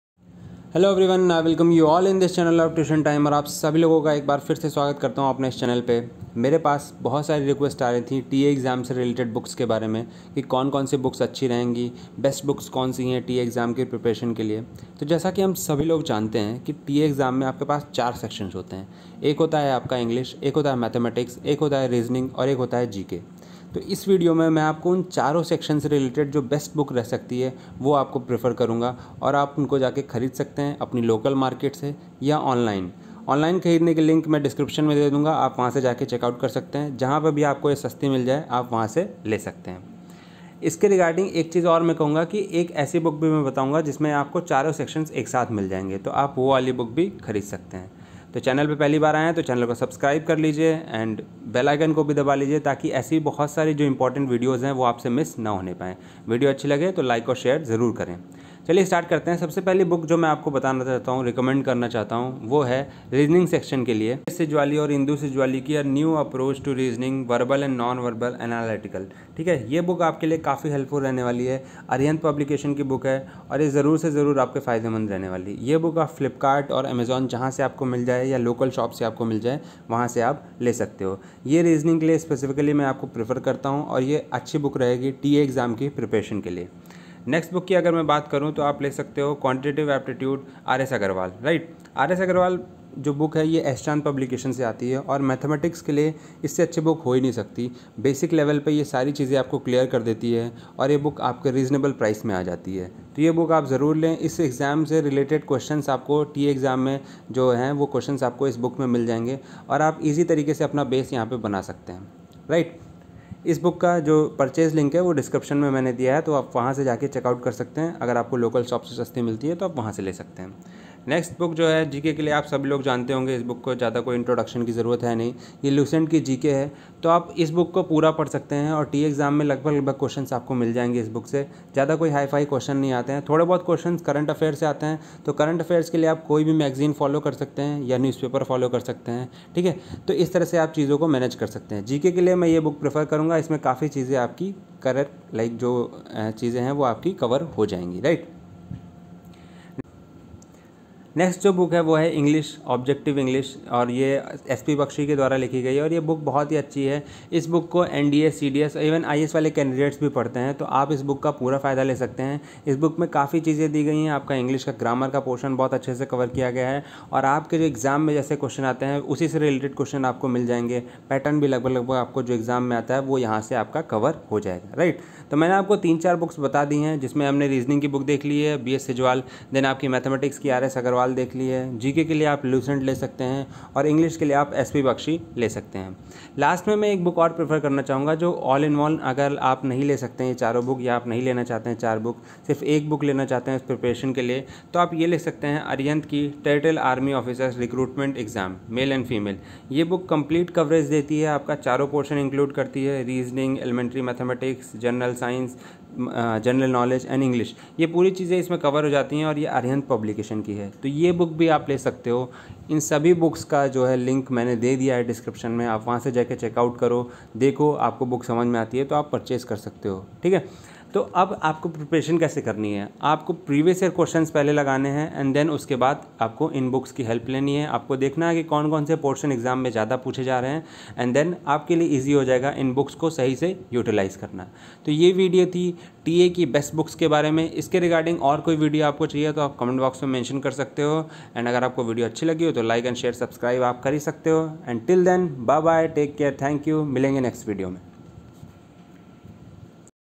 BEST BOOKS FOR TERRITORIAL ARMY EXAM PREPARATION 2020,TA EXAM 2020 ,
BEST BOOKS FOR TERRITORIAL ARMY  EXAM PREPARATION 2020
BOOKS :

Arihant Territorial army book
Territorial Army Officers Recruitment

BOOKS FOR SSB INTERVIEW (ARMY , NAVY AIRFORCE )